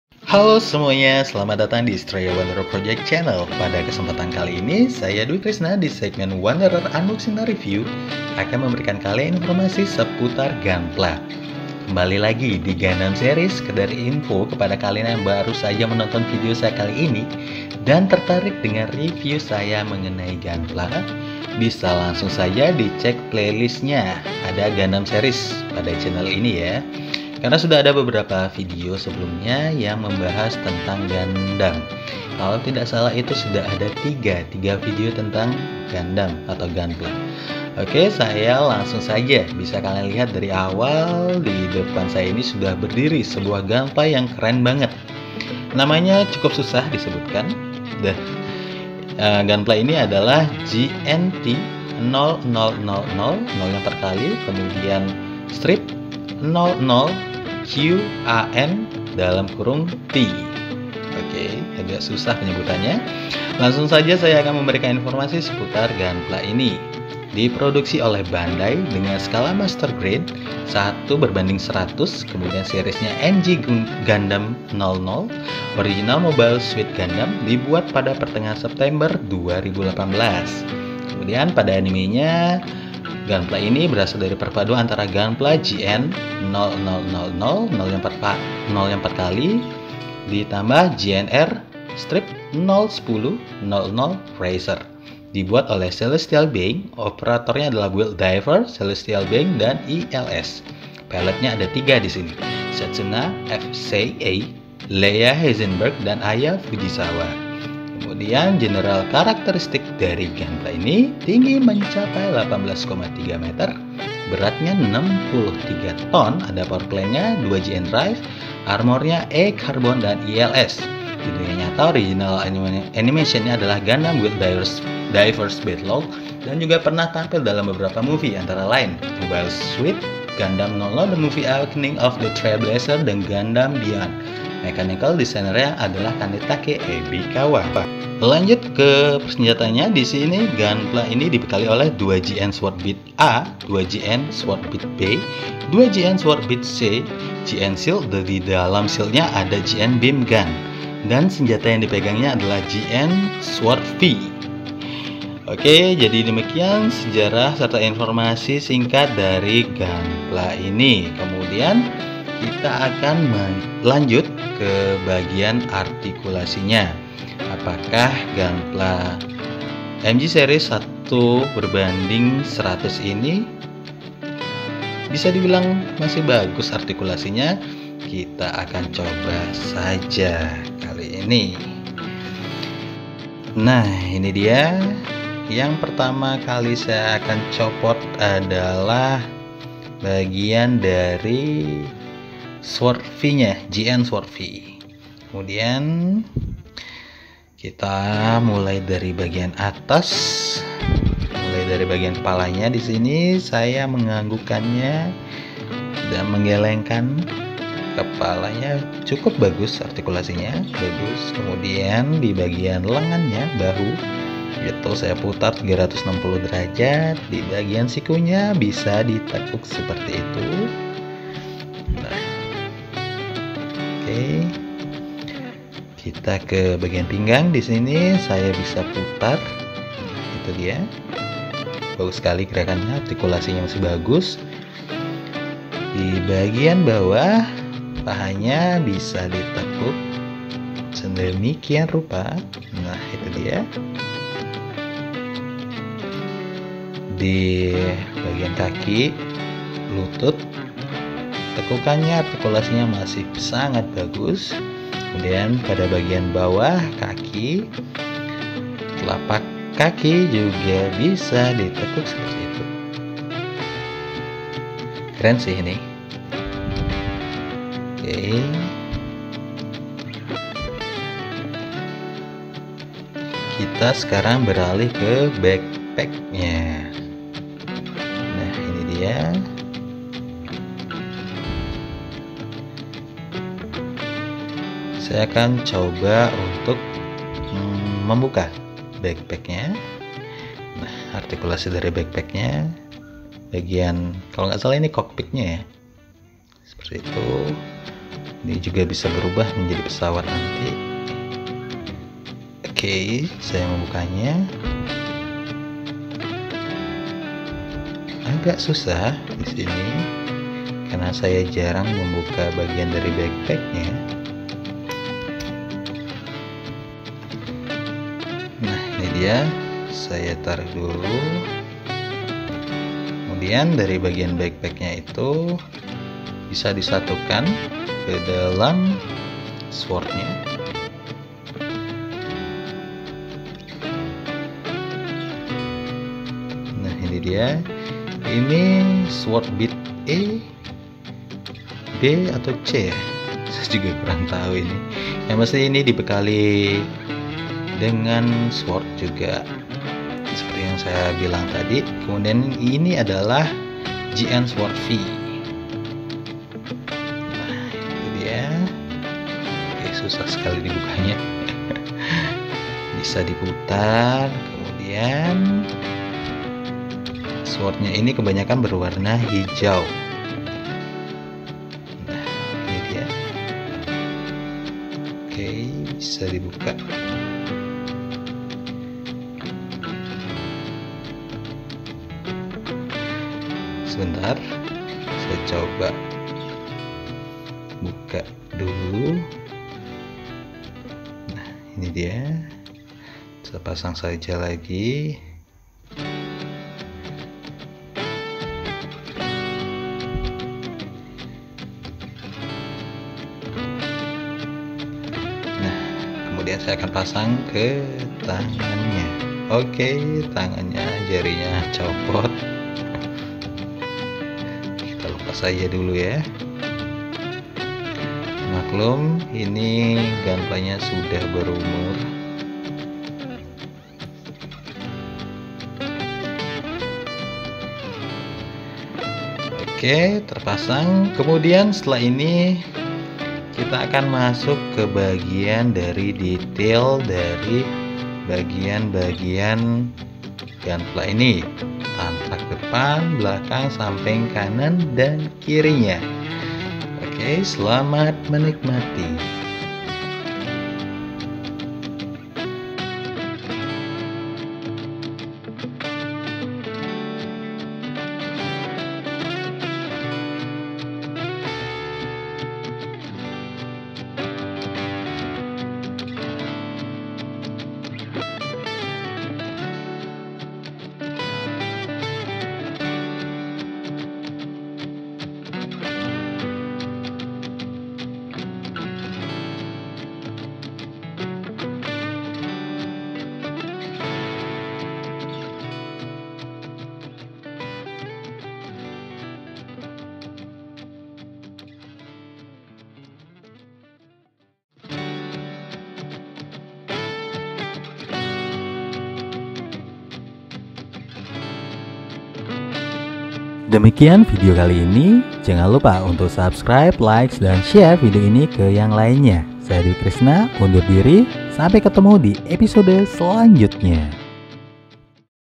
MG 1 : 100 GNT 0006 - QAN (T) | Bahasa Indonesia | Gundam Series | Unboxing & Review Eps. 26
Pada segmen Wanderer Unboxing & Review kali ini, saya akan memberikan review dan informasi singkat tentang Gunpla GNT 0006 - QAN (T) . Tetap ikuti episode dan seriesnya, karena masih banyak lagi Gunpla yang akan direview.